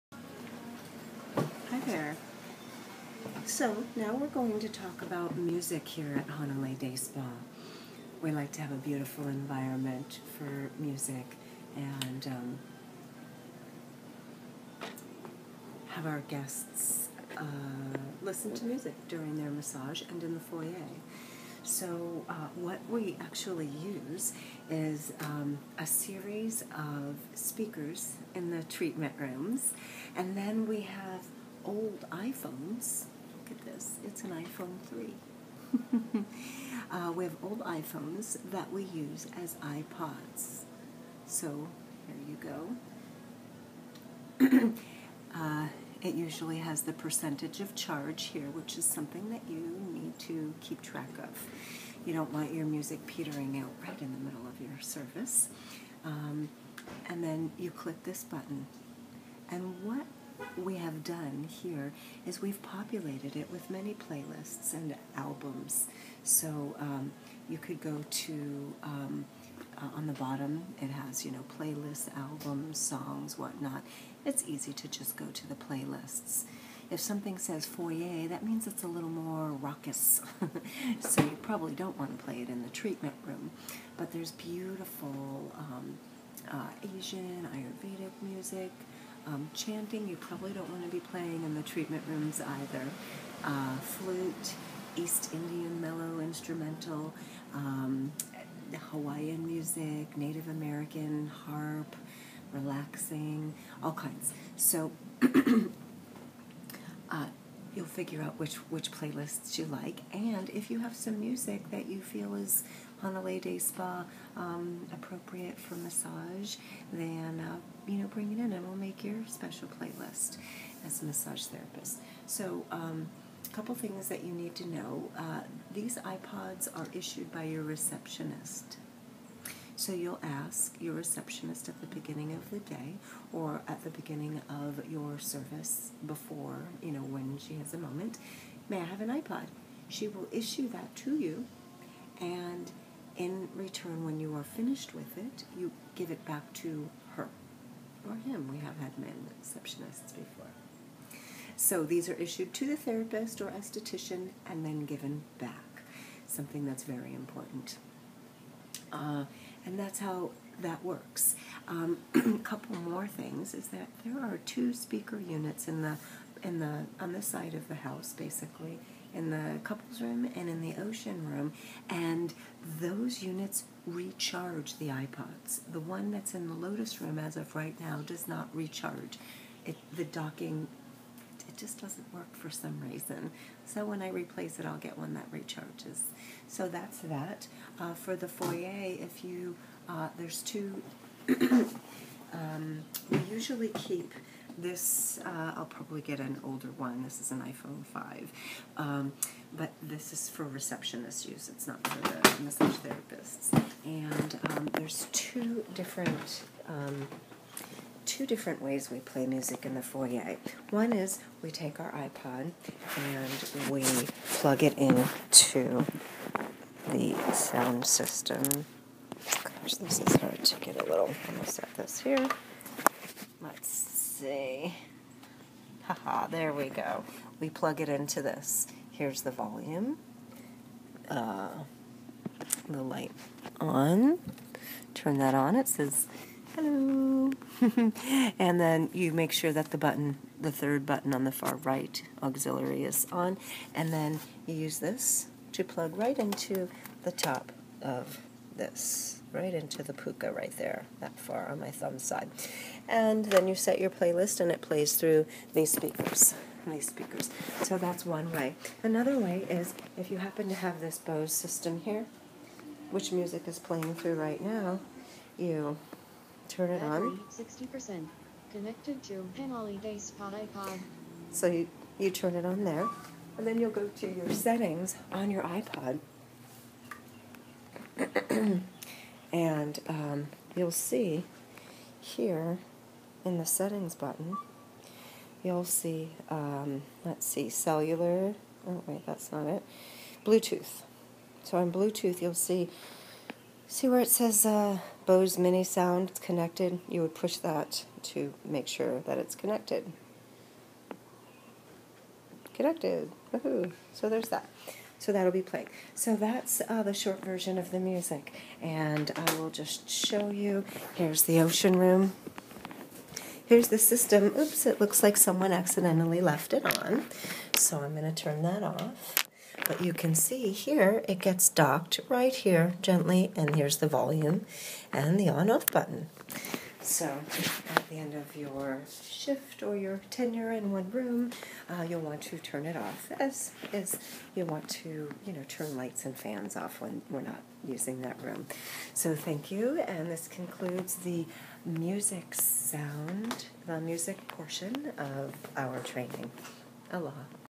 For all employees: spa and massage music
Music at Hanalei Day Spa, in treatment room and the foyer

iPods are issued directly to the therapist from the receptionist
and handed back to the receptionist.

All treatment rooms have speakers, right now, only couples and ocean room charge the iPods on the dock

make sure your iPod has enough charge
remember to turn off your speakers at the end of the service
*click the light off to save battery (on top of the iPod)